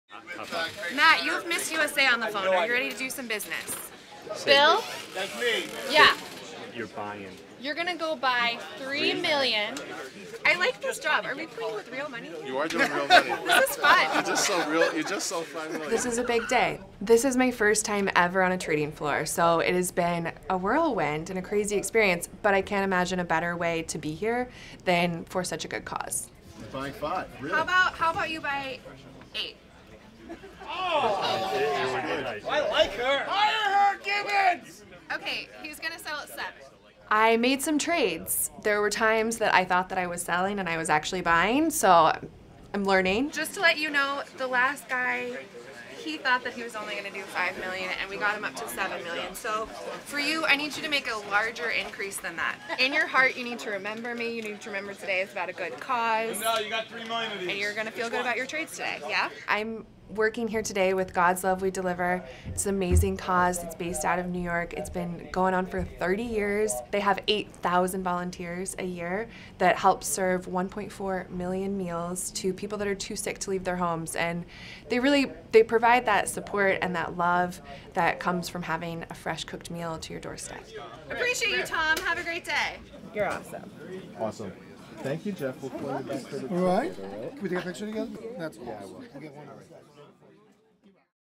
Miss USA (Olivia Jordan) Reigns on Wall Street: Cantor Fitzgerald Charity Day 2015
Every year, Cantor Fitzgerald and our affiliate, BGC Partners, in conjunction with the Cantor Fitzgerald Relief Fund, commemorate our 658 friends and colleagues and 61 Eurobrokers employees who perished on September 11, 2001 by distributing 100% of our global revenues on Charity Day to the Cantor Fitzgerald Relief Fund and dozens of charities around the world. Since its inception, Charity Day has raised approximately $125 million globally. Each anniversary is a poignant occasion for us. Charity Day is our way of turning a tragic day into one that is positive and uplifting by helping others.

Joining us this year was Miss USA 2015, Olivia Jordan, representing God's Love We Deliver. God's Love We Deliver is a non-profit charity organization based in New York City which deliver meals to home bound people with HIV/AIDS, cancer, and other serious illnesses.

This year Charity Day was held on Friday, September 11, 2015. We raised approximately $12 million, and donated 100% of our global revenue to over 100 charities worldwide.